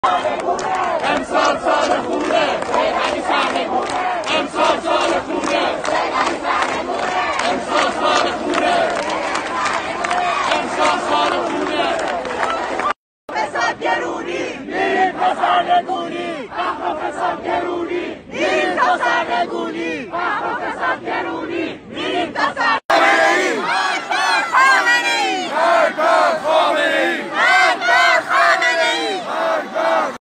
فقر ، فساد، گرونی می‌ریم تا سرنگونی/ دانشگاه پلی‌تکنیک ۱۶ آذر
دانشجویان پلی‌تکنیک: فقر ، فساد، گرونی میریم تا سرنگونی ۱۶  آذر ۱۴۰۱